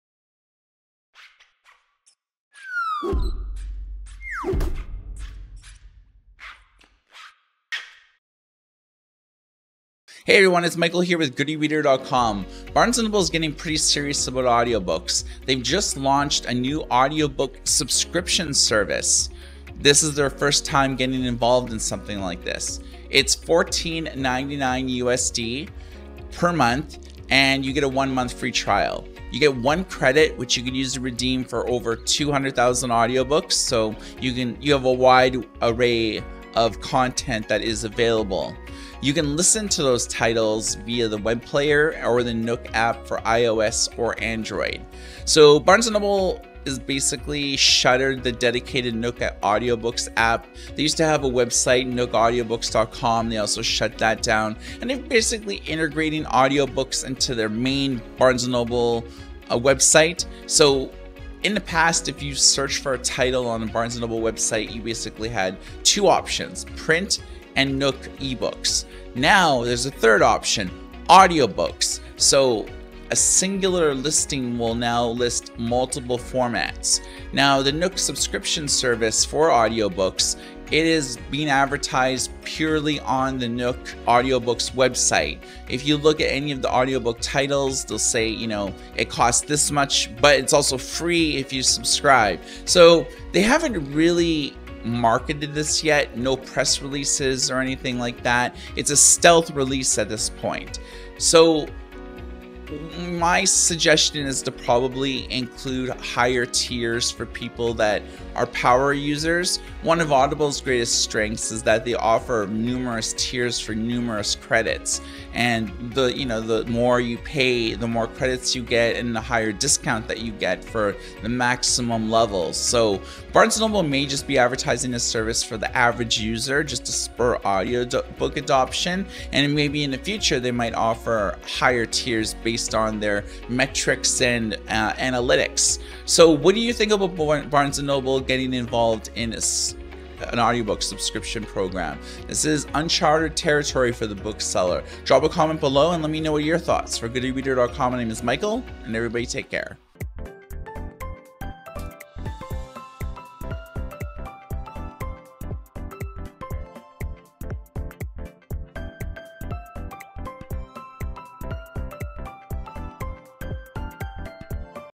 Barnes & Noble Nook Audiobooks Subscription Service | Good News
Nook has a new Audiobook Subscription service!

Michael explains all that is going down.